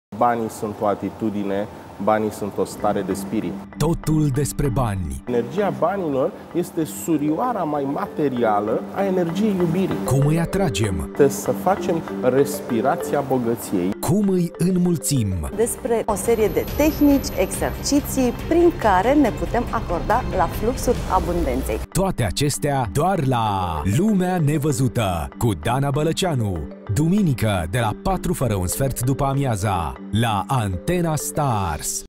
Cum sa devii un magnet pentru bani!
Maestrul Risvan Vlad Rusu prezintă, duminica, de la ora 15 45, la Antena Stars, o serie de tehnici simple pentru a atrage banii și prosperitatea în viata noastră!